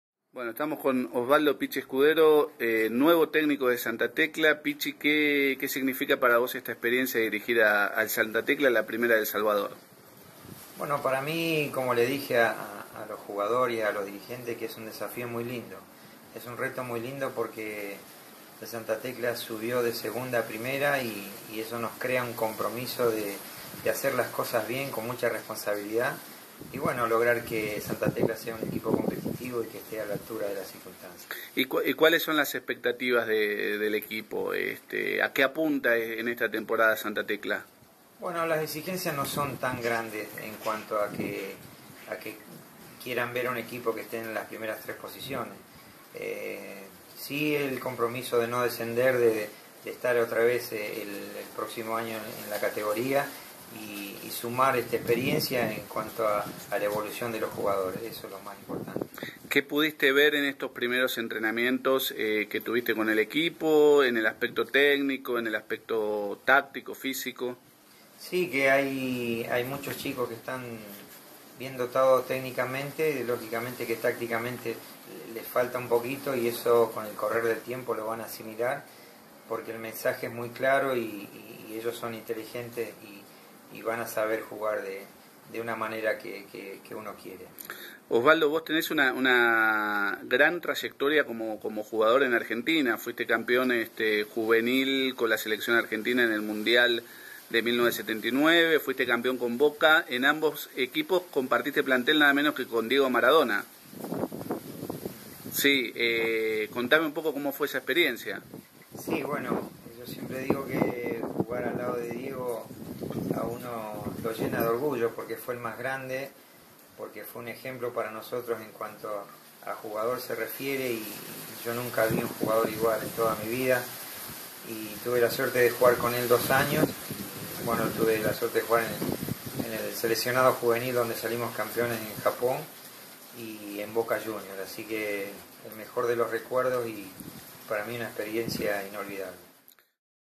El nuevo técnico del Santa Tecla
El "Pichi" escudero es uno de los nuevos protagonistas del fútbol salvadoreño. Sobre su espalda lleva una extensa carrera en el fútbol argentino. Ahora toma las riendas del CD Santa Tecla en su aventura en la Primera División